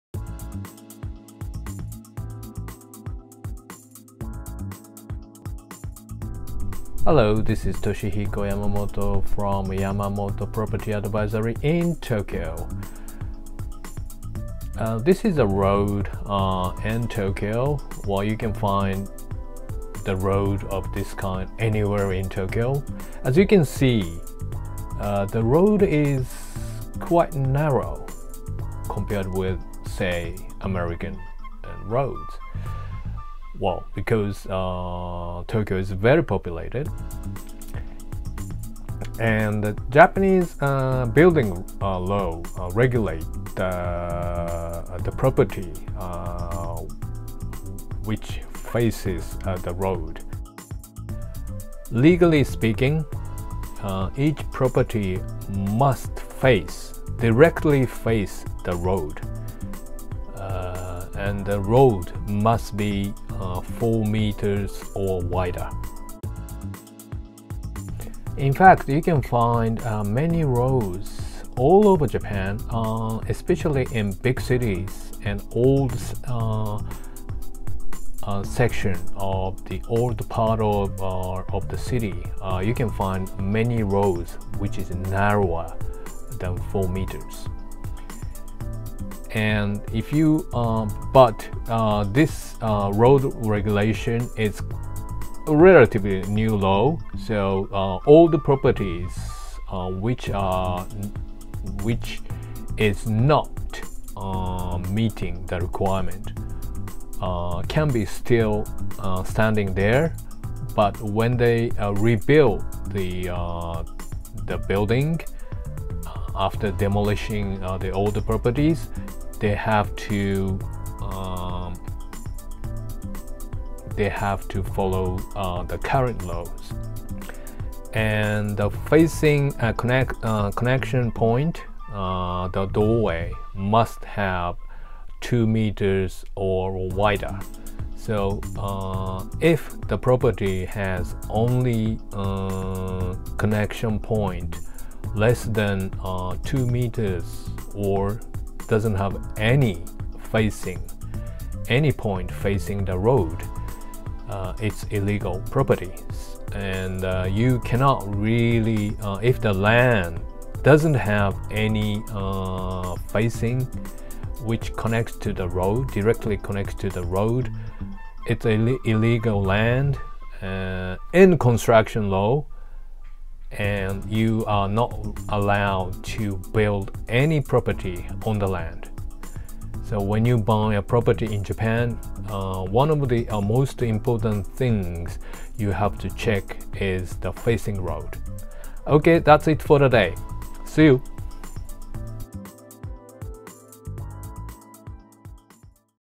Japanese construction laws on the facing road of the property
Japanese construction laws (building laws) strictly regulate the roads
which properties face.
Generally, roads must have at 4 meters wide and a land lot must have direct facing (path way) of minimum 2 meters wide. If it is less than 2 meters, it is illegal to build on this block.
But in reality, there a number of old properties which are not meeting the regulations because those old houses were built before such strict regulations were introduced.